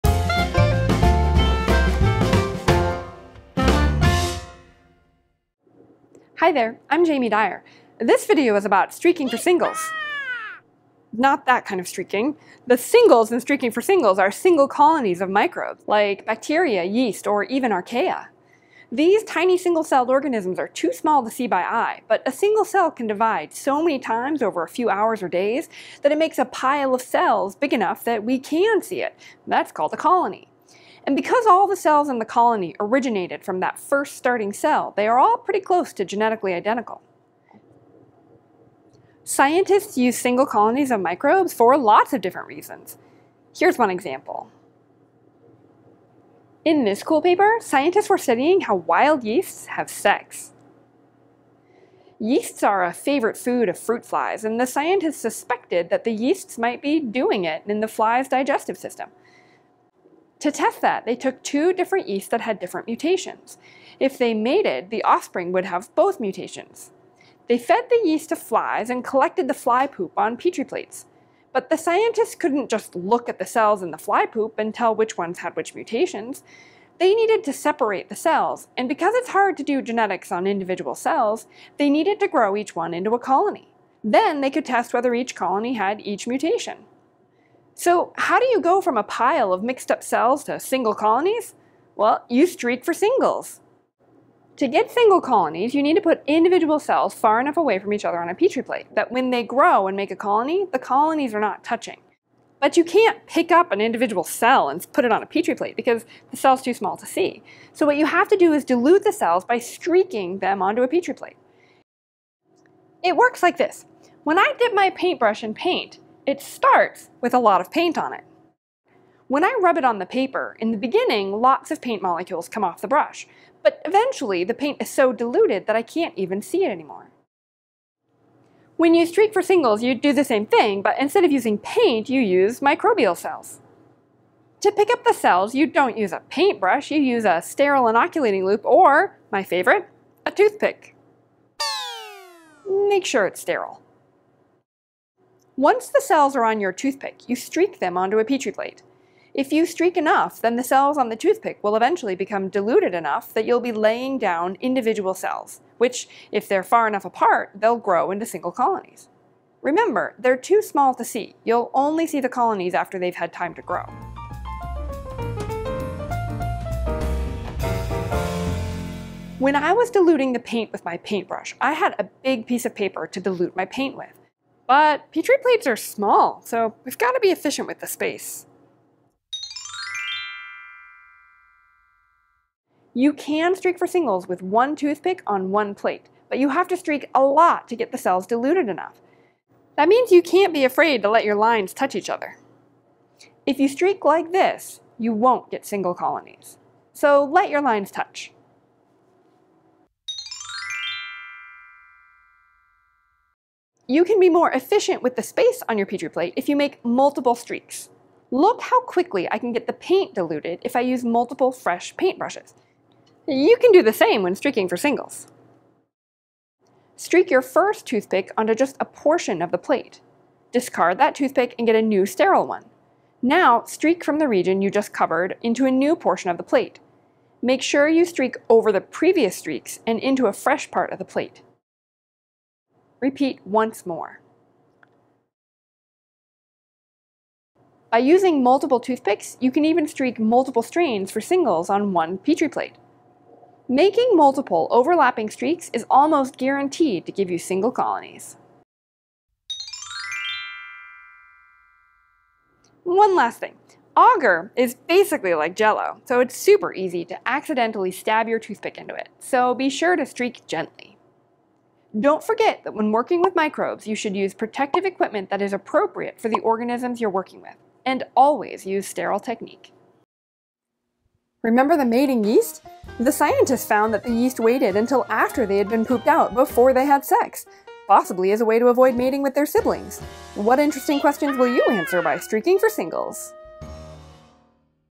How do you Streak for Singles? A practical guide!
What do yeast sex and streaking have to do with microbiology? Find out in this video that explains why scientists streak for singles (hint: it's to separate microbes on a petri plate) and provides practical tips to streak for singles in the lab.

The research paper about yeast sex:
A. W. McClure, K. C. Jacobs, T. R. Zyla, D. J. Lew, Mating in wild yeast: delayed interest in sex after spore germination. Molecular Biology of the Cell 29, 3119–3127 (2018).

Scientific Consultation by Kathy Zarilla
Cartoon Illustrations by Michelle Lotker

Want YouTooBio in your Inbox? Monthly Newsletters with new videos and teaching tips for the Biology classroom!